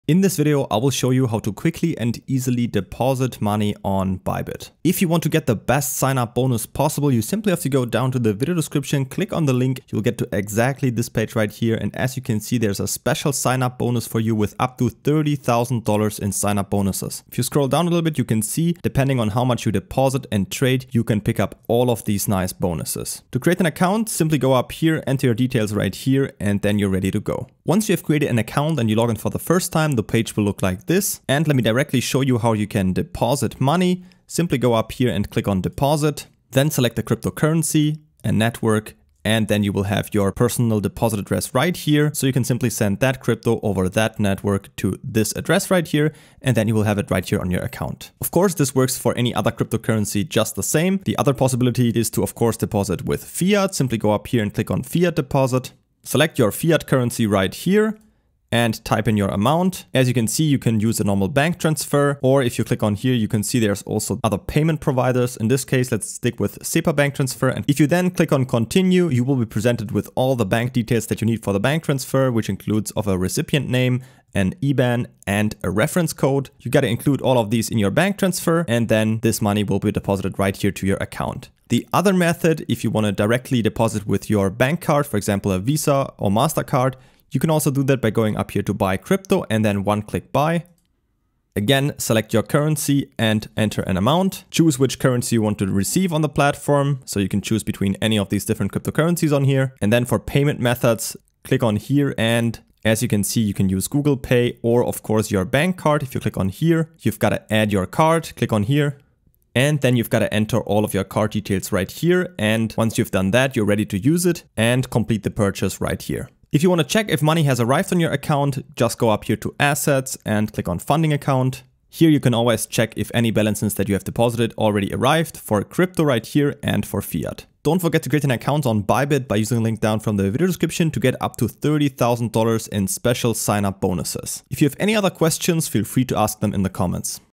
Deposit Money on Bybit ✅ Deposit Tutorial (Fiat & Crypto)
⏱️Timestamps⏱️
00:00 Create account (Up to $30,000 Bonus)
00:30 How to deposit Crypto (BTC, USDT, ...)
00:50 How to deposit Fiat (EUR, USD, ...)
02:08 Pick up to $30,000 Bonus